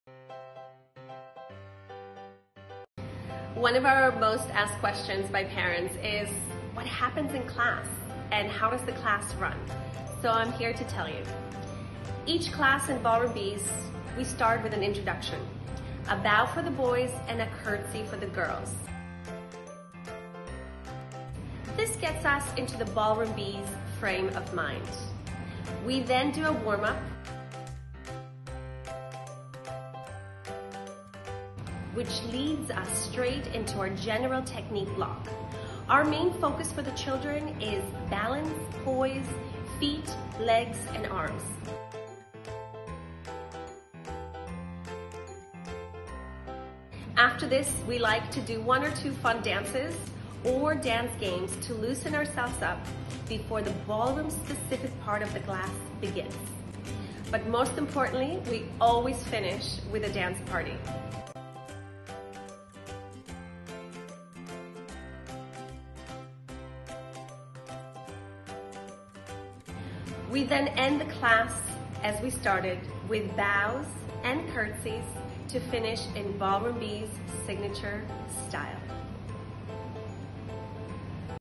Early childhood learning - Dance
Hello everyone- my name is Katya Virshilas and I am the founder of BallroomBees.
Our name is BallroomBees and we do take our inspiration from ballroom dancing, but we are early childhood learning dance classes.
Children especially until 4 yrs have to learn a lot of general technique before we specialise them. In other words before we can learn the Waltz we need to learn to bend and straighten our knees, weight transfer with control and to flex and point our feet. In our BabyBees class (1.5-2.5 yrs)we focus on gross motor skills and developing their senses, most of the dances are recognizable children's songs and dances, and everything is very play based. As our bees continue to grow the classes grow with them. We start to introduce partner work, and our continued focus is on gross motor skills and musicality. These are the building blocks for learning to dance.
Most importantly we love to see our bees leave the class full of inspiration and love of dance!

@ballroombees have 6 locations across Hong Kong